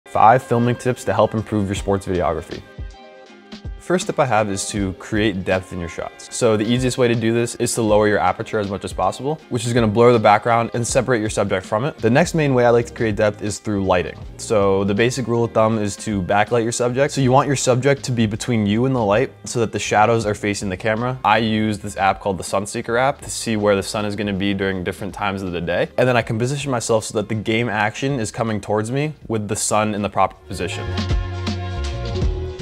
How to Create Depth: Sports Videography Tip
This is part 1 in a 5 part series sharing filming tips to make better sports videos. Be sure to follow along for the next part!